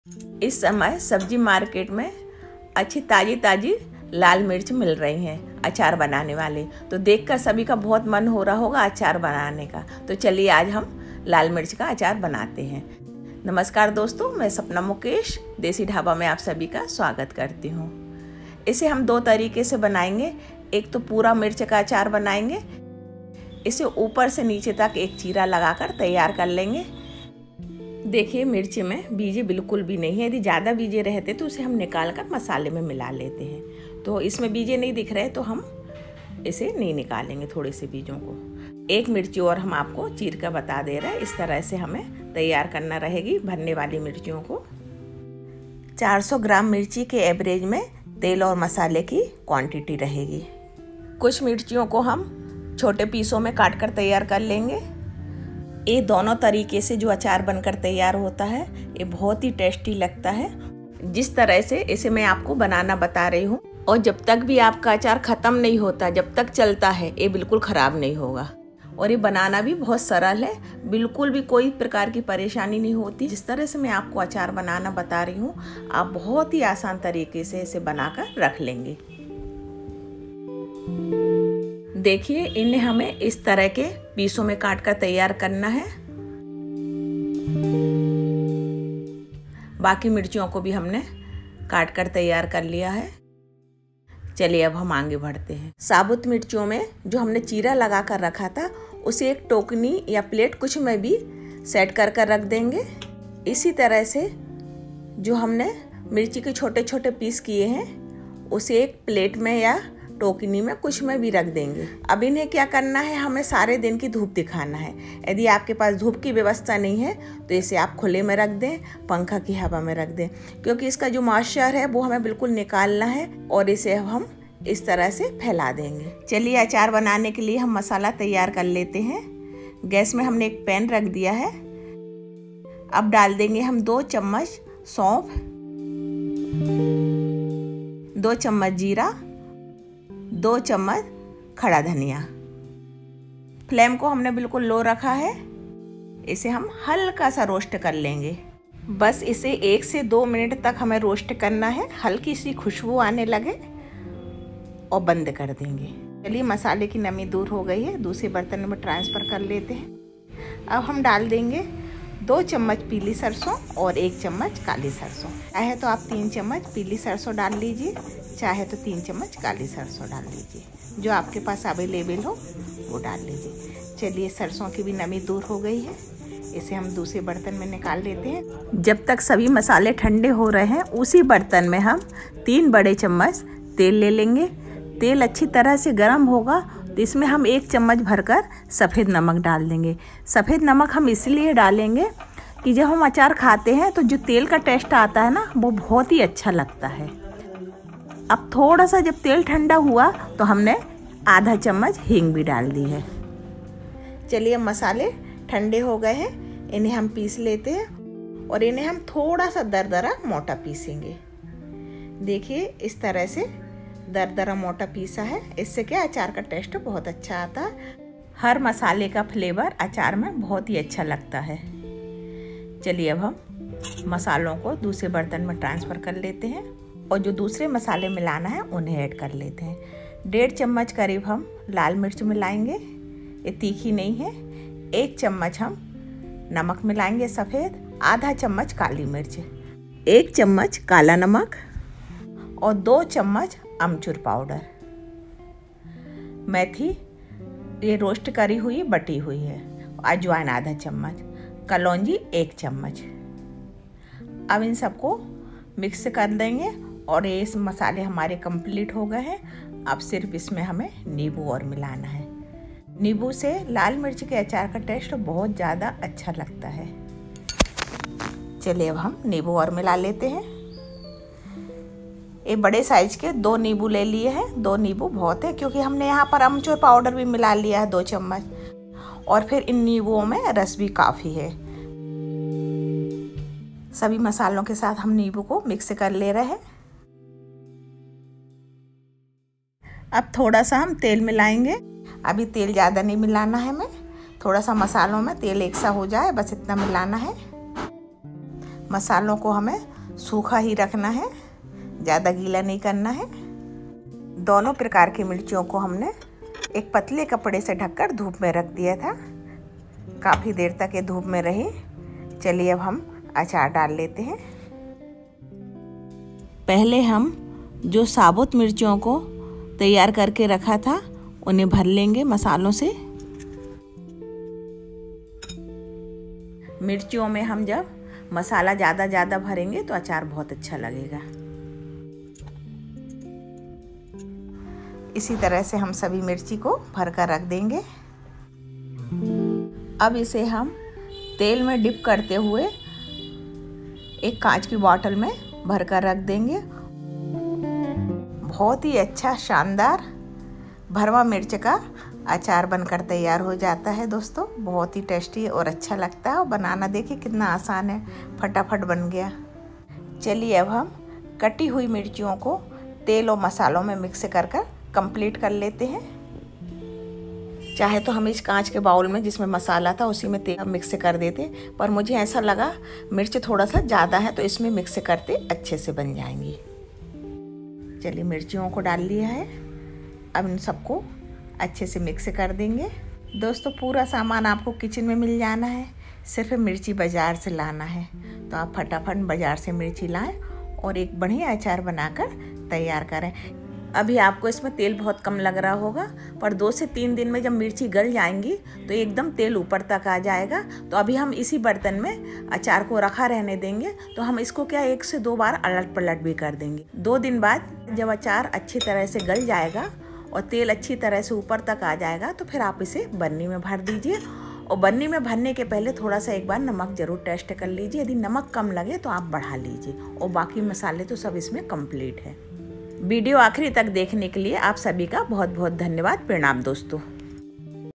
ताज़ी मोटी लाल मिर्च का सरल विधि से हमने बनाया अचार moti lal mirch achar resipe॥ desi dabha
Ingredients
400 g.m. M Lal mirch
 2 tsp zeera
2 tsp fennel सोफ़
2 tsp coriander धनिया
2 tsp  kashmiri chilli powder
1 tsp turmeric powder
1black साल्ट
 1 tsp methi dana
1/2tsp kali mirch powder
2tsp amachur powder
3tsp साल्ट salt
3 midim oil 300 g.m.
1tsp kaloji